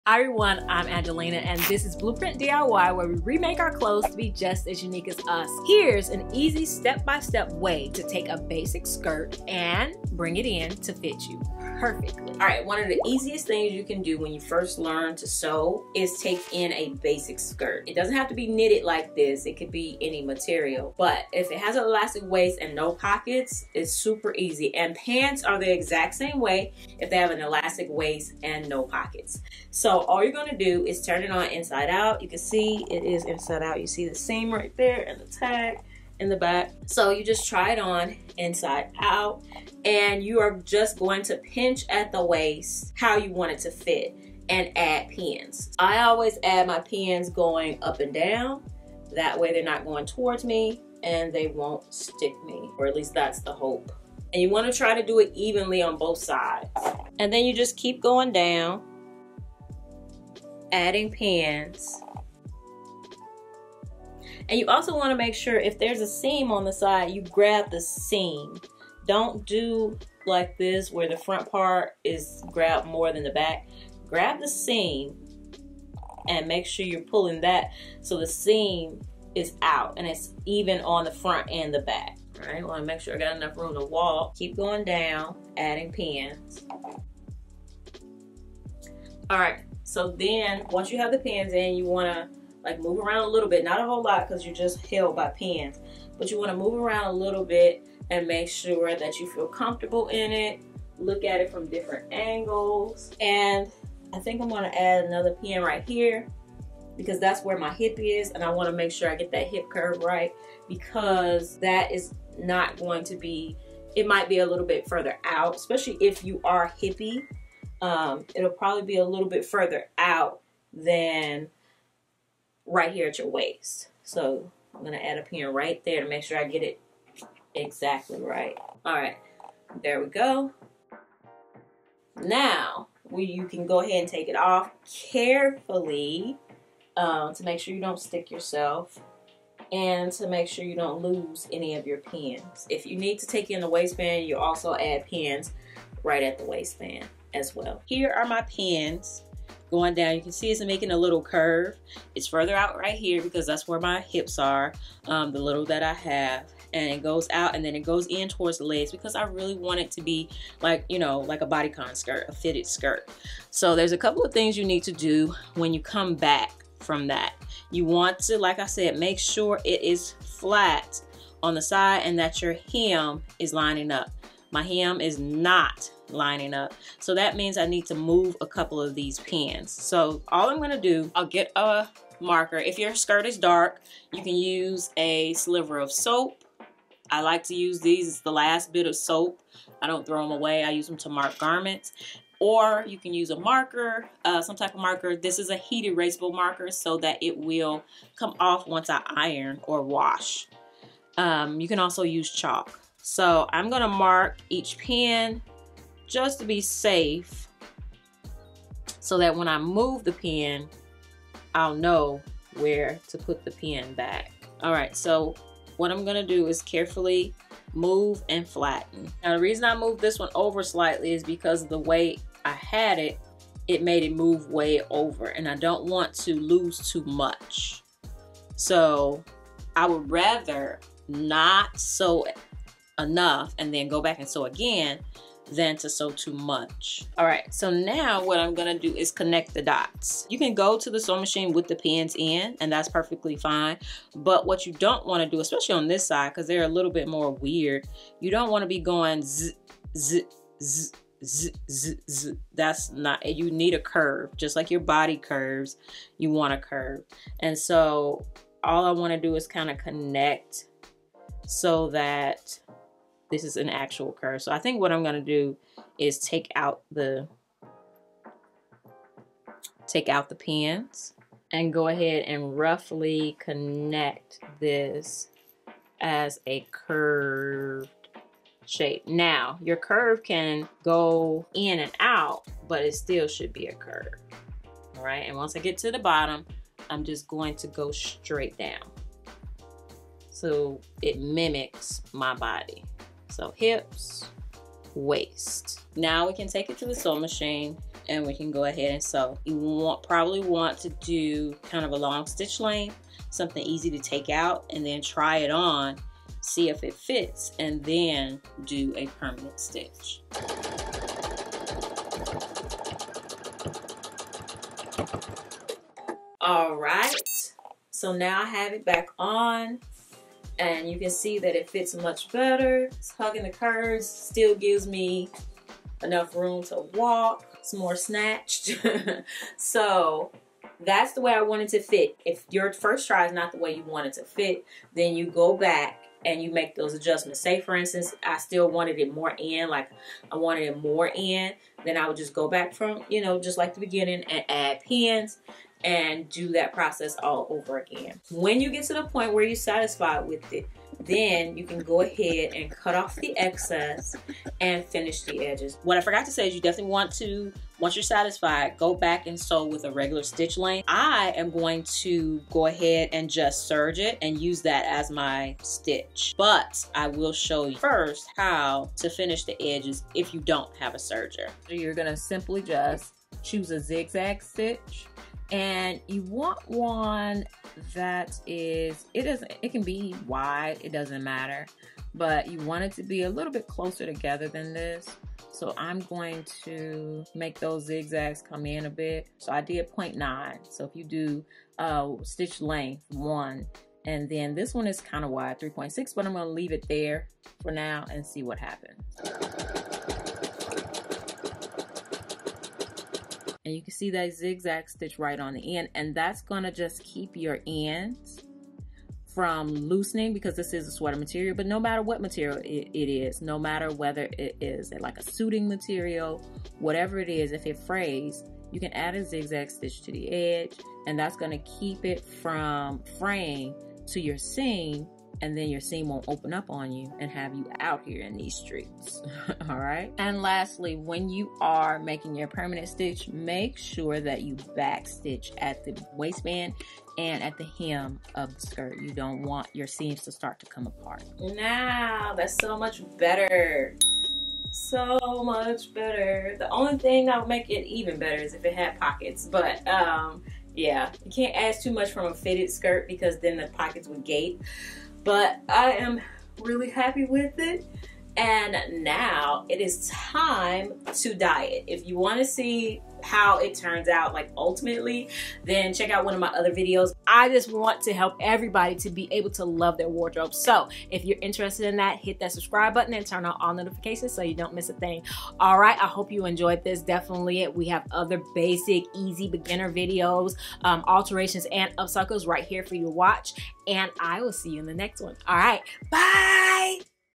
How To Take In A Skirt EASY! | Making My Clothes Fit part 2
Here's the easiest way to take in a basic skirt or pants using your sewing machine step by step for my beginner sewist.

Remake your wardrobe to be just as fun as you! Upcycle your clothes to fit your size and personality with a little creativity and a few alterations. Join this crew of refashion renegades!